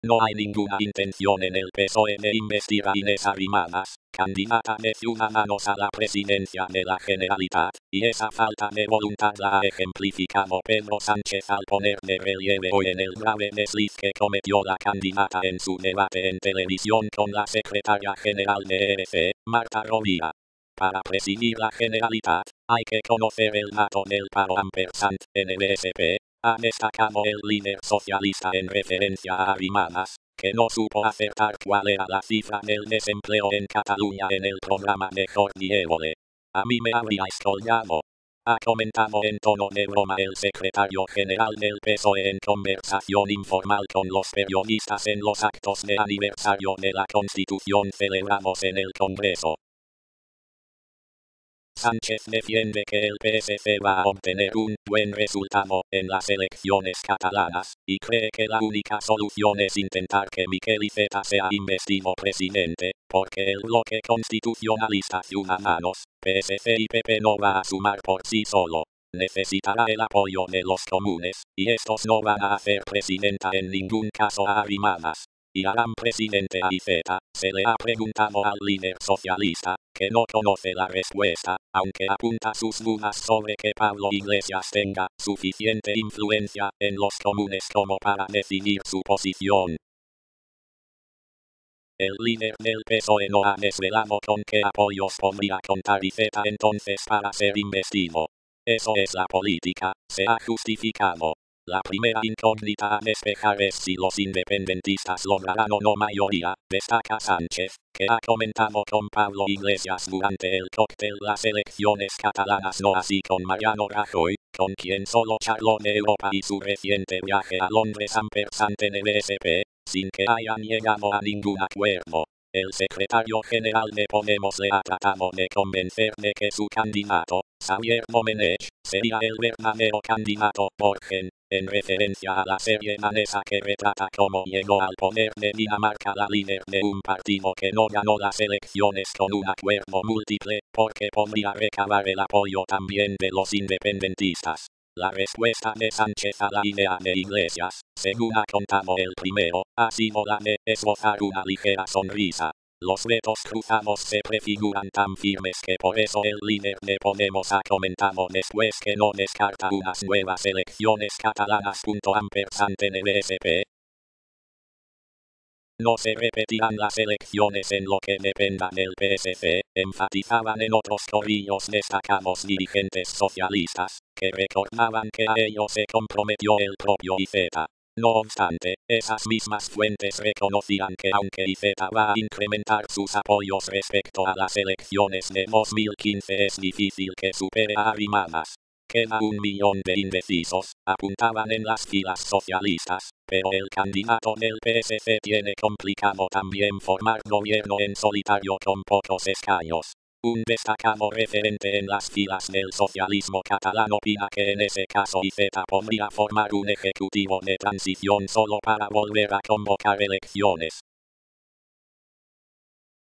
Sánchez, sobre Arrimadas: “Para presidir la Generalitat, hay que conocer el dato del paro”
No hay ninguna intención en el PSOE de investir a Inés Arrimadas, candidata de Ciudadanos a la presidencia de la Generalitat, y esa falta de voluntad la ha ejemplificado Pedro Sánchez al poner de relieve hoy en el grave desliz que cometió la candidata en su debate en televisión con la secretaria general de ERC, Marta Rovira. "Para presidir la Generalitat, hay que conocer el dato del paro",&nbsp; ha destacado el líder socialista en referencia a Arrimadas, que no supo acertar cuál era la cifra del desempleo en Cataluña en el programa de Jordi Évole. "¡A mí me habríais colgado!", ha comentado en tono de broma el secretario general del PSOE en conversación informal con los periodistas en los actos de aniversario de la Constitución celebrados en el Congreso. V: ves_000988.mp4